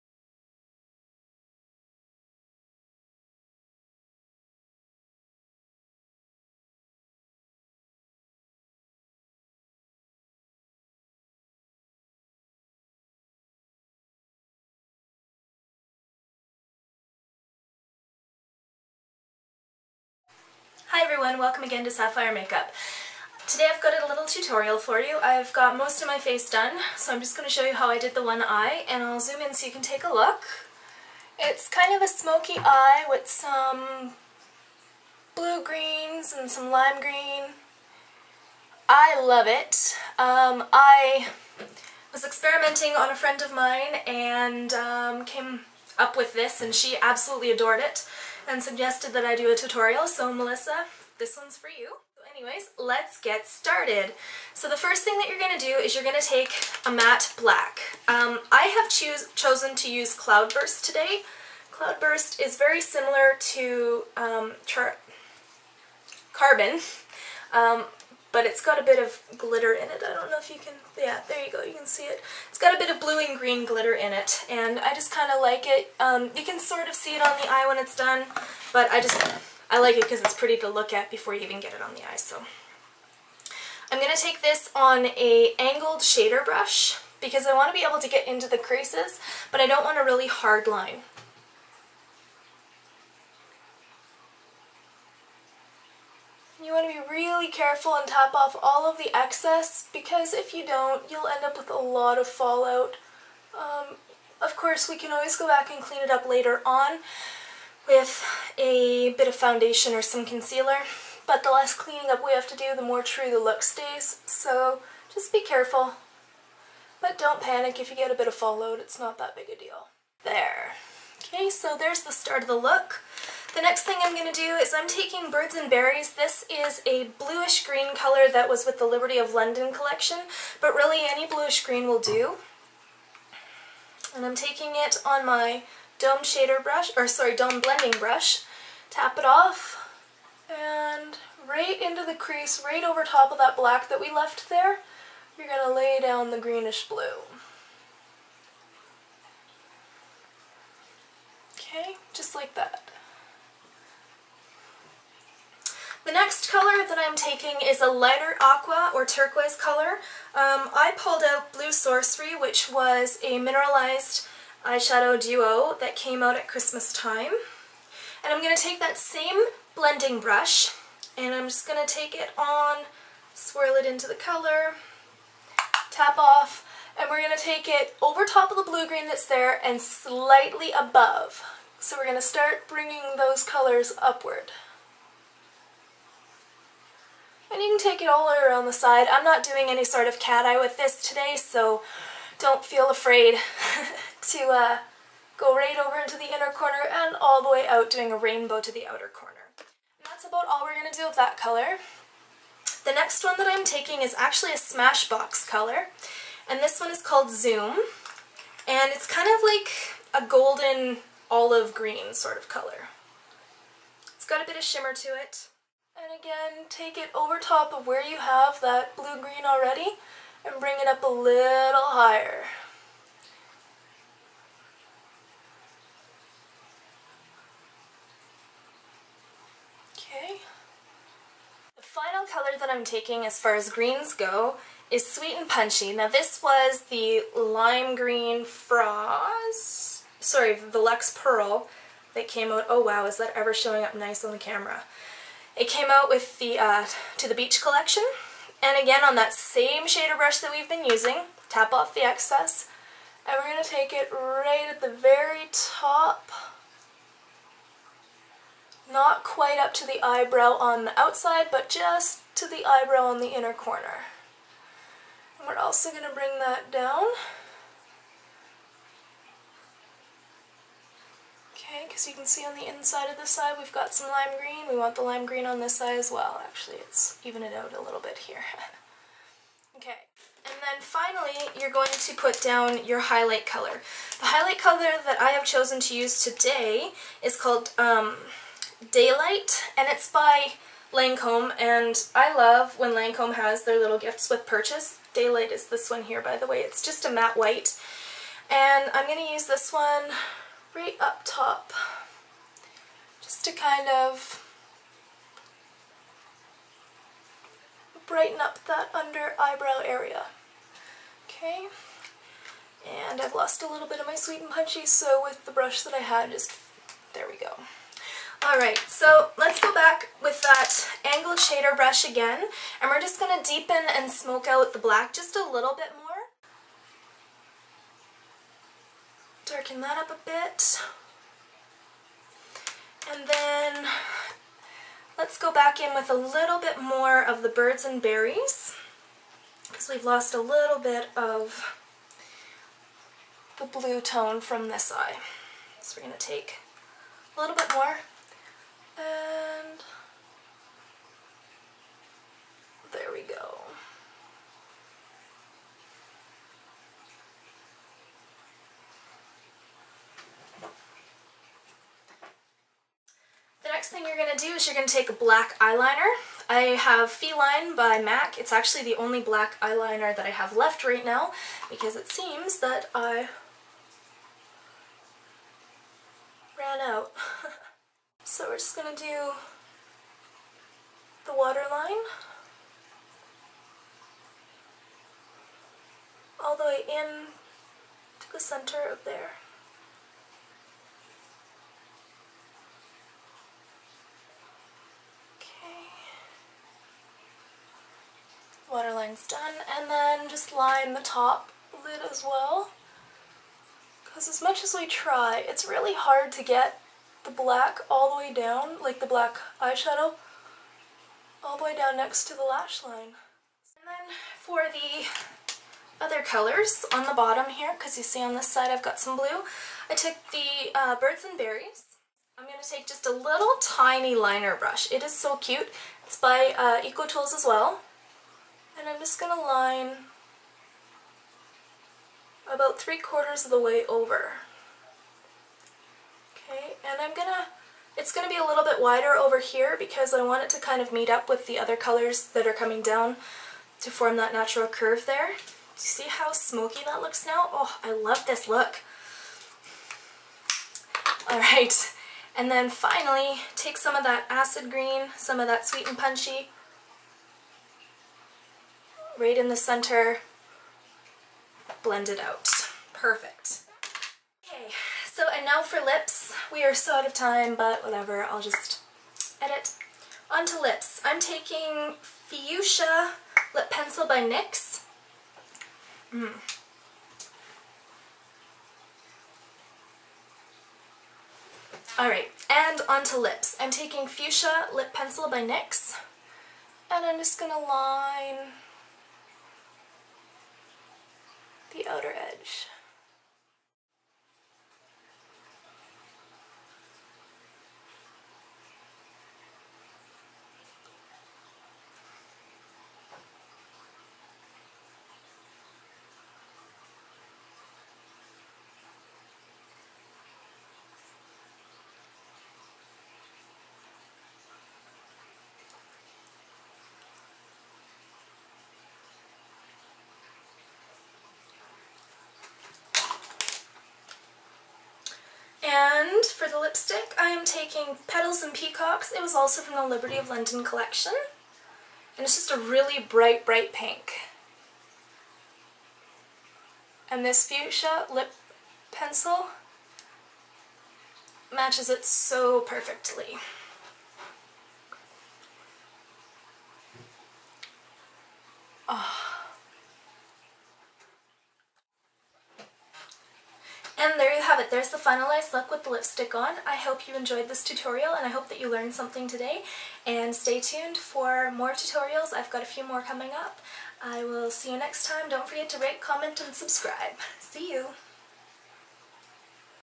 Peacock Smoke
This is a look I did on a friend of mine a couple of months ago.

NYX and L'Oreal have many inexpensive dupes for most of the MAC products mentioned in this video.
All products used were purchased with my own money. All opinions are my own.